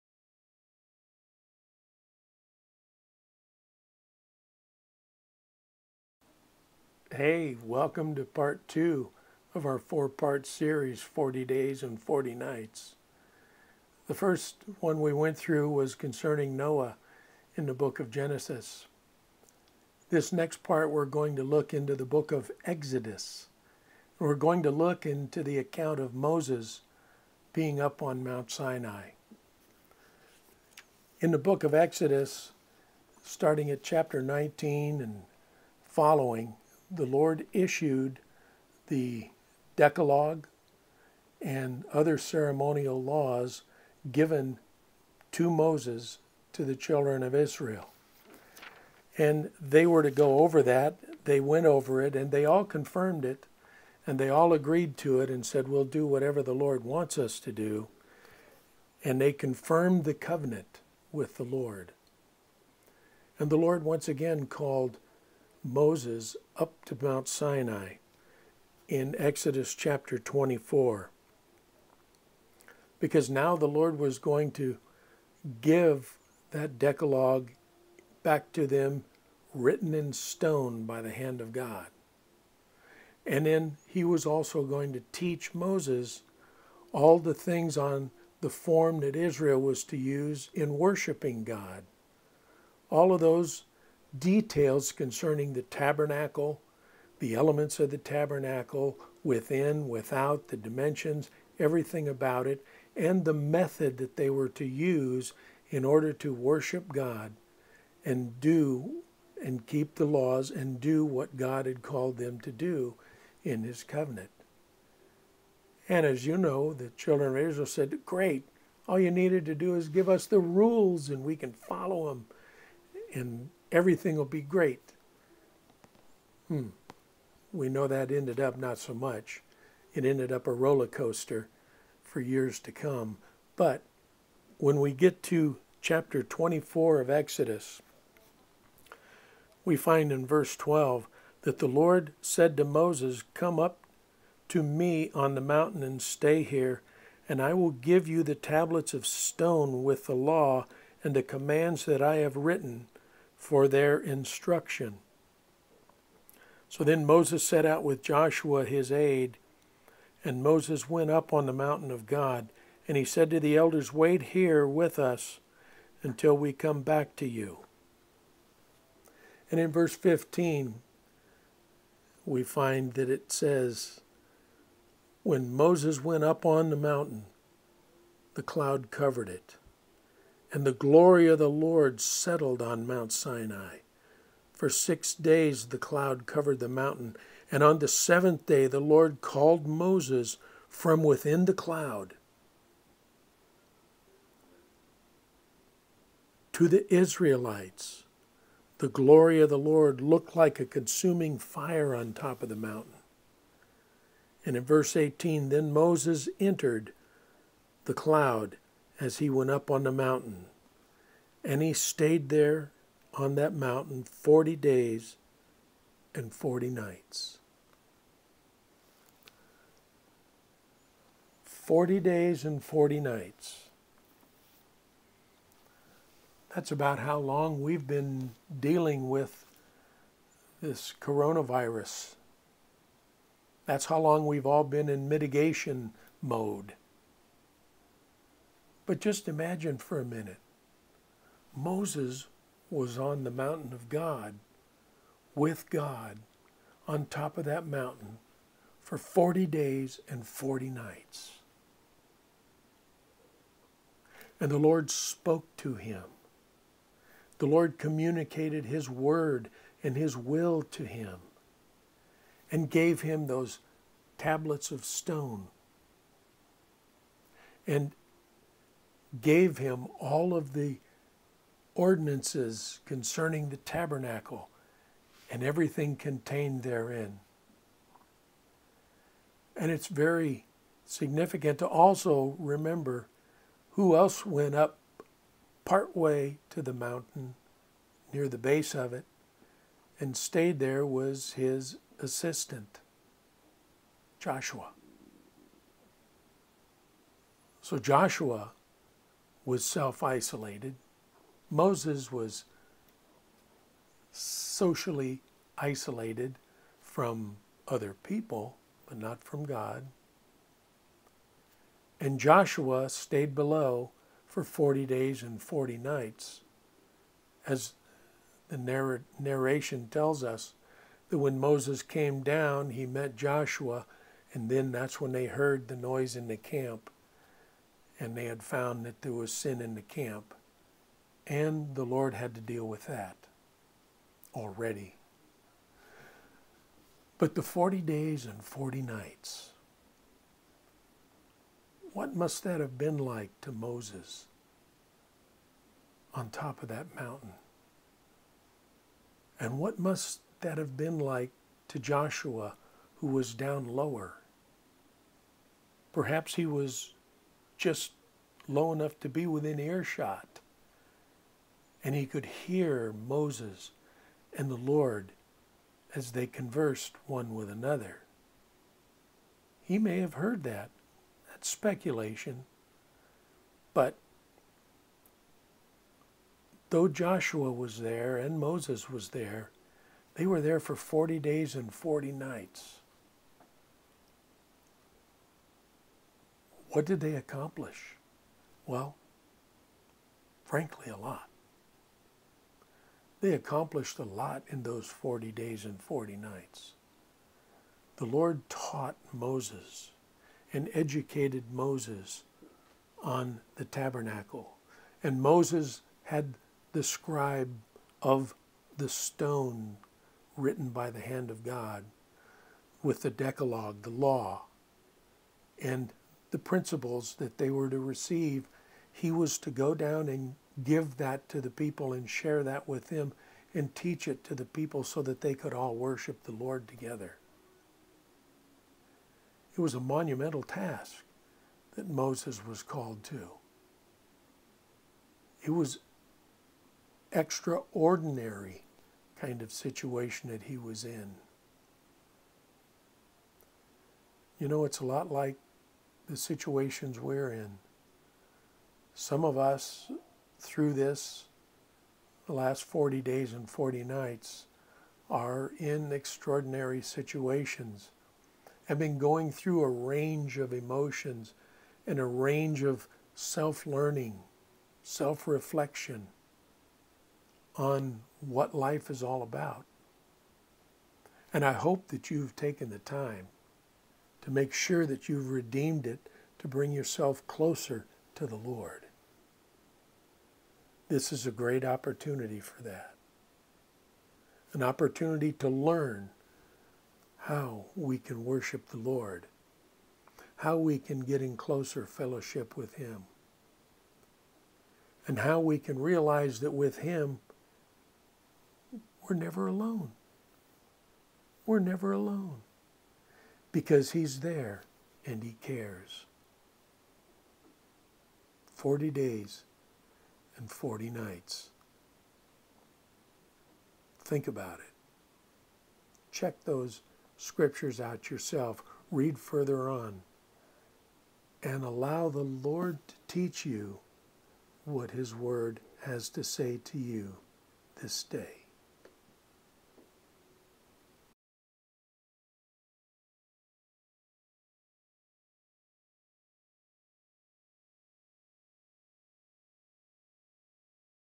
40 Days and 40 Nights (Part 2)
40 Days and 40 Nights (Part 2) in the journey of Self Isolation in the Covid-19 Mitigation Process. In this part, a Biblical Perspective is focused on David - when he brought a sling and stones to a sword fight after 40 days and 40 nights of being bullied as God's people. Blessings be yours!